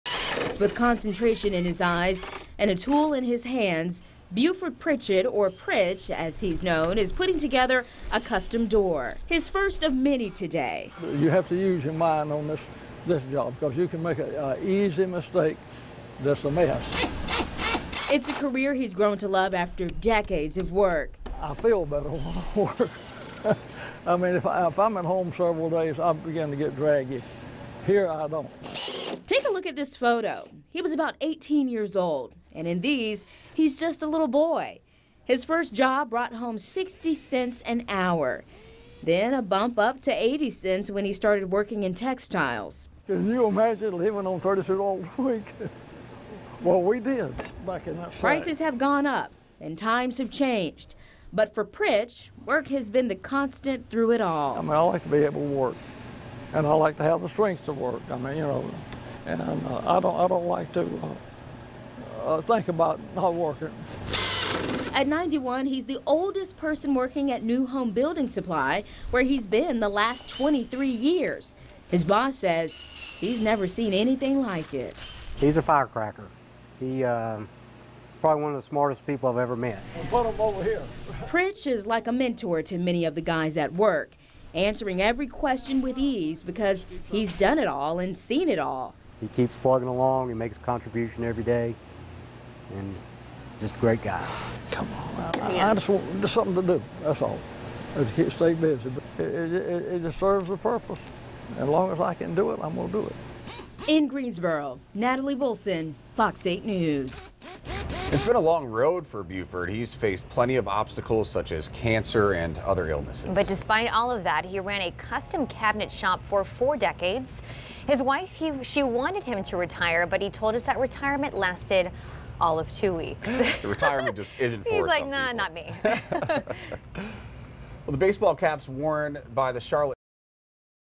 91-Year-Old North Carolina Man Still Working, Says Age Won't Stop Him
For 40 years Buford Pritchett ran a custom cabinet shop in North Carolina. Now, he's the oldest person working at New Home Building Supply. Pritchett has been there for the last 23 years and is a mentor to many of the guys at work.